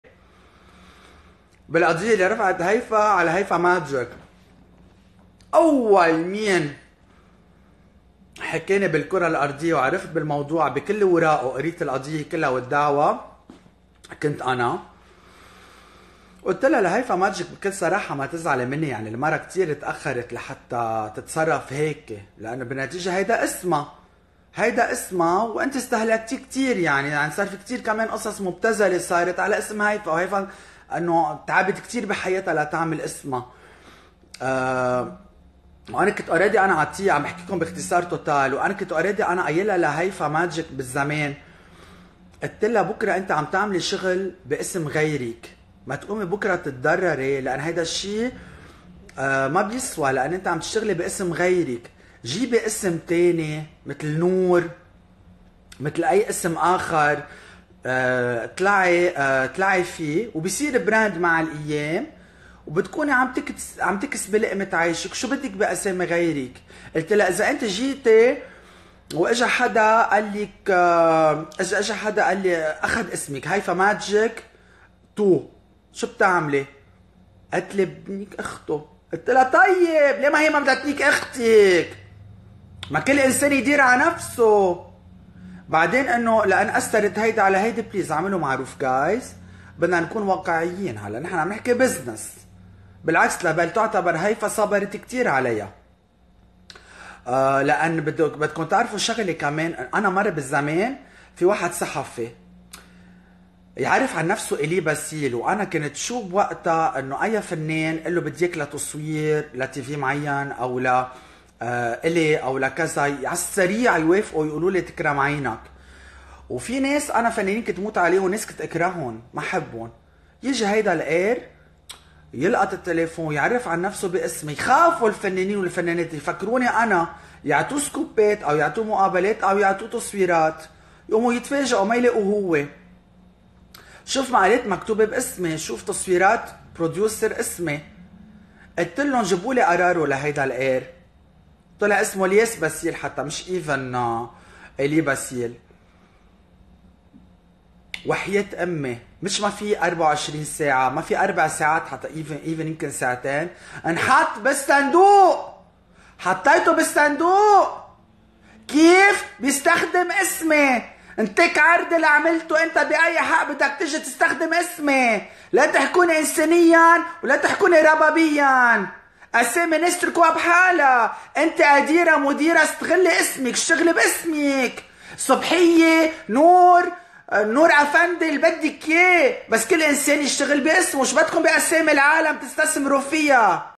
ايلي باسيل يعلق على دعوى هيفا وهبي ضد هيفا ماجيك
الصحافي المثير للجدل ايلي باسيل يعلق على الدعوى التي قدمتها هيفاء وهبي بحق هيفا ماجيك
شاهدوا الفيديو ولا تنسوا الاعجاب والتعليق ومشاركة الفيديو والاشتراك بالقناة ليصلكم كل جديد